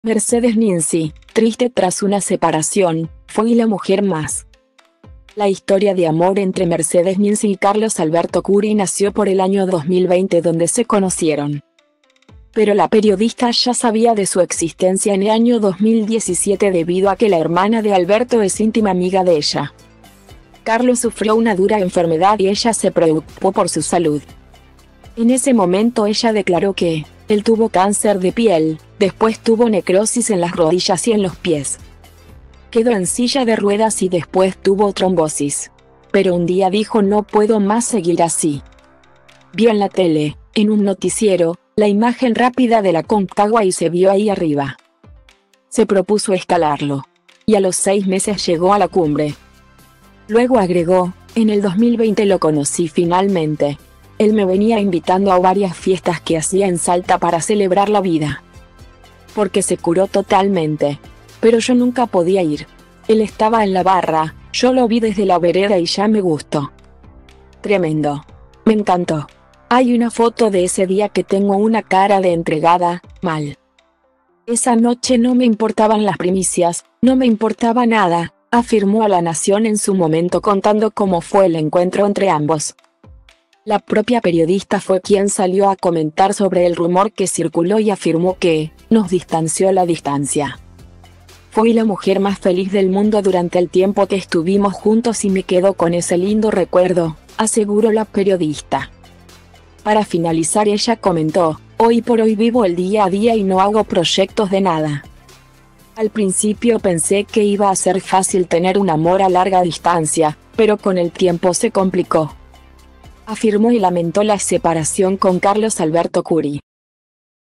Mercedes Ninci, triste tras una separación: “Fui la mujer más…”ArGH2
Mercedes Ninci, triste tras una  separación: “Fui la mujer más…”
La historia de amor entre Mercedes Ninci y Carlos Alberto Curi nació por el año 2020 donde se conocieron.Pero la periodista ya sabía de su existencia en e año 2017 debido a que la hermana de Alberto es íntima amiga de ella.Carlos sufrió una dura enfermedad y ella se preoucpó por su salud.